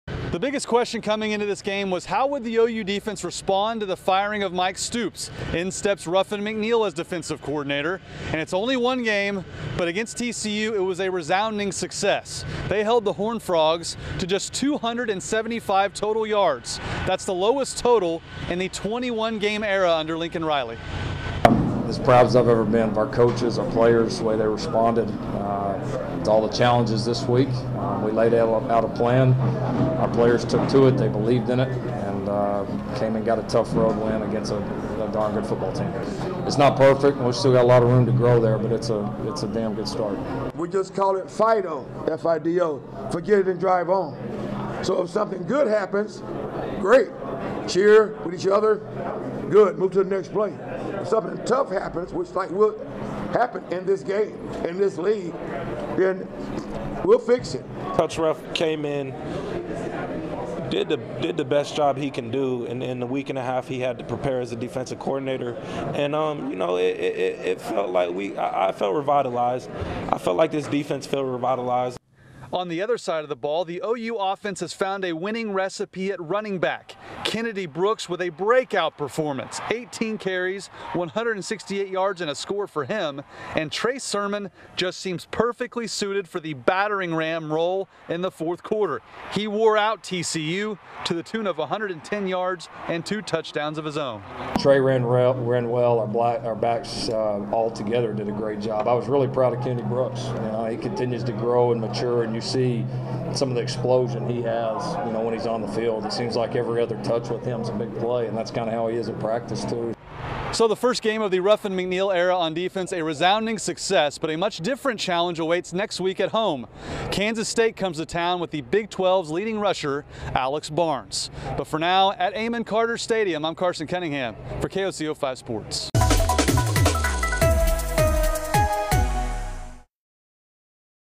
Defense makes statement in first game without Mike Stoops
The Oklahoma defense put up a solid performance during the team’s first game since the Sooners parted ways with now-former defensive coordinator Mike Stoops.